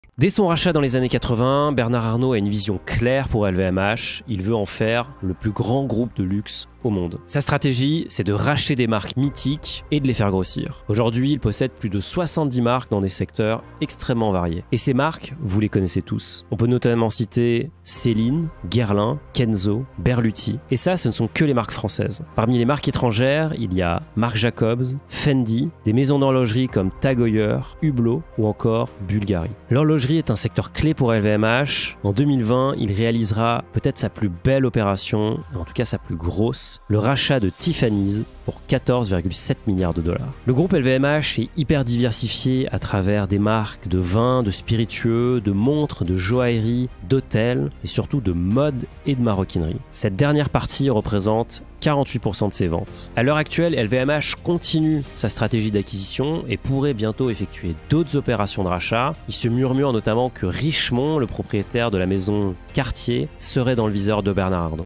L'ascension de Bernard Arnault: de l'acquisition de LVMH à l'empire du luxe Source: Finary
Bernard Arnault a acquis LVMH dans les années 80 avec la vision de devenir le plus grand groupe de luxe au monde. Sa stratégie consiste à racheter des marques emblématiques et à les développer. Aujourd'hui, LVMH possède plus de 70 marques dans divers secteurs, dont Céline, Guerlain, Kenzo et Berluti. En plus des marques françaises, il détient des marques étrangères telles que Marc Jacobs, Fendi, Tagoyer, Hublot et Bulgari. L'horlogerie est un secteur clé pour le groupe.